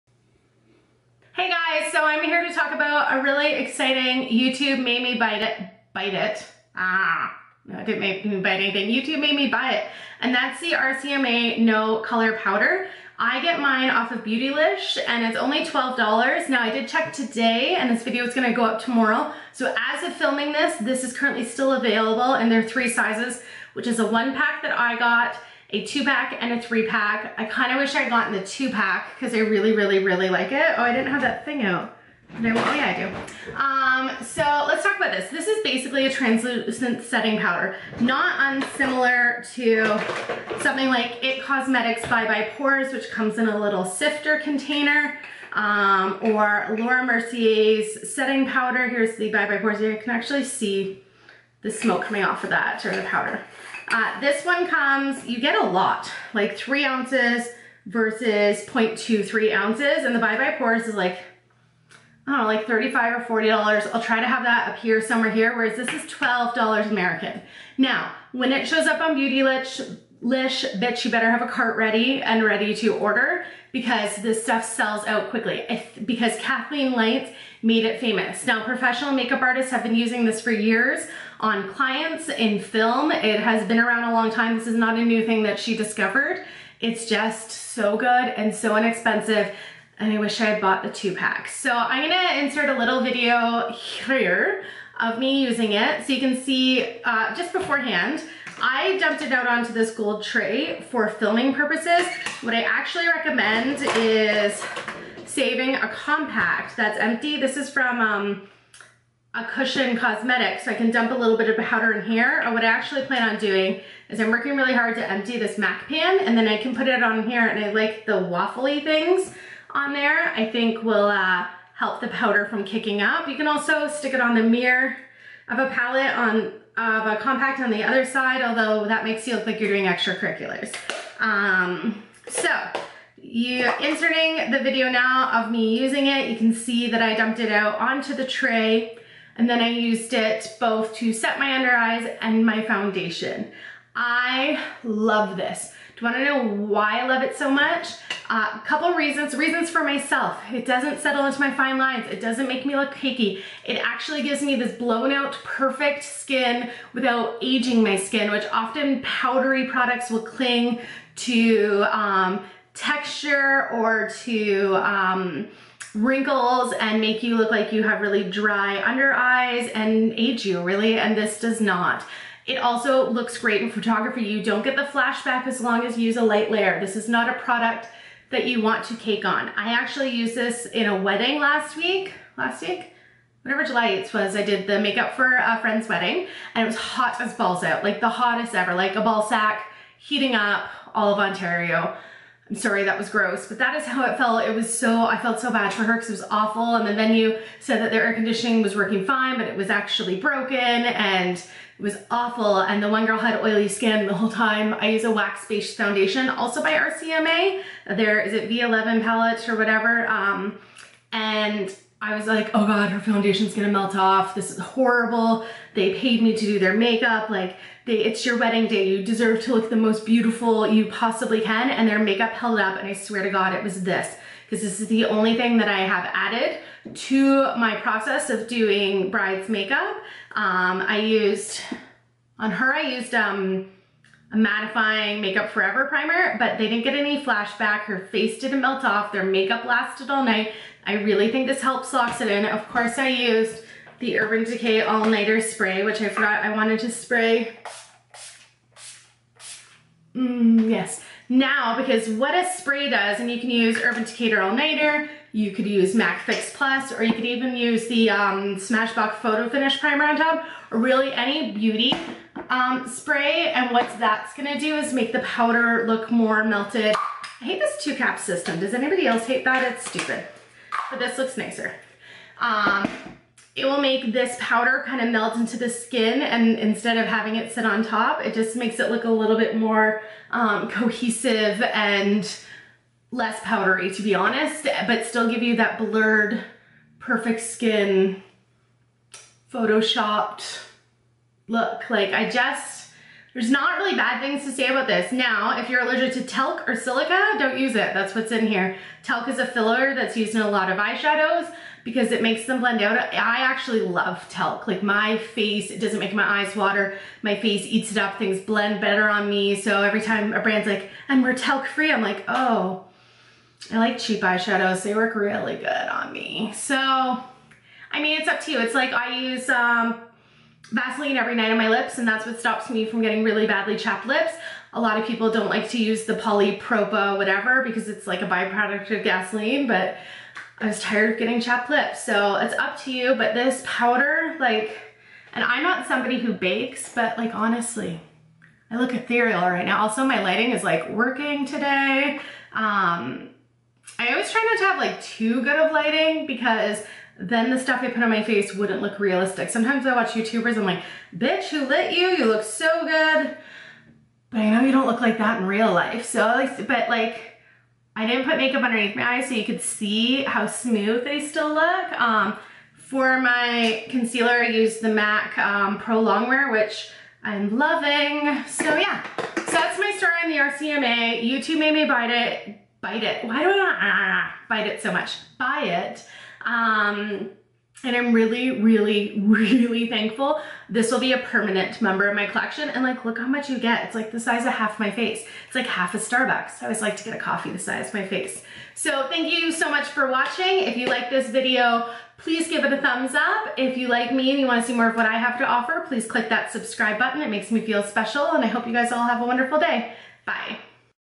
BEST POWDER EVER | RCMA No Color Powder : YouTube Made Me Buy It
Thank God YouTube made me buy this powder, I will never not own this.  I grabbed mine at beautylish